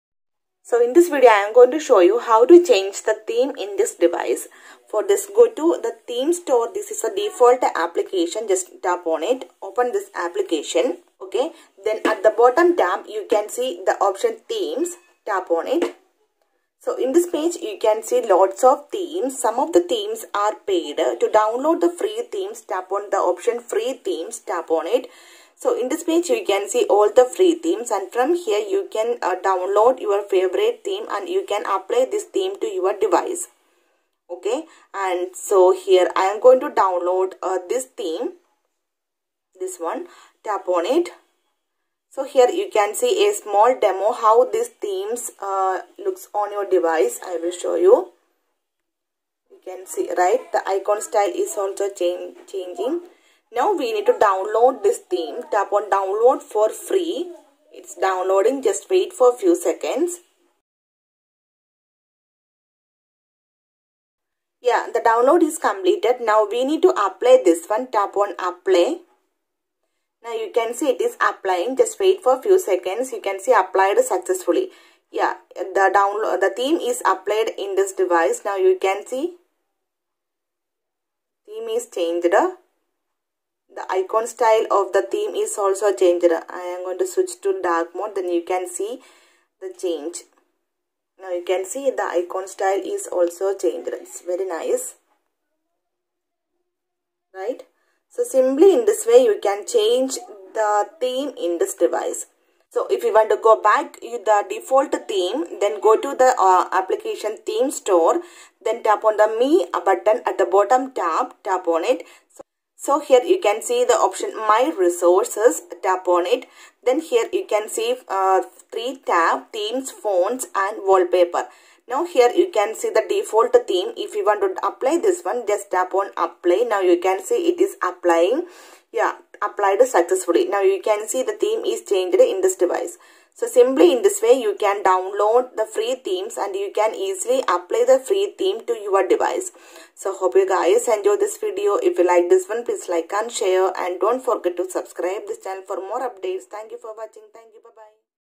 Realme C21 | Change Theme in Realme C21
Hi friends this video shows you that " How to change theme in Realme C21 "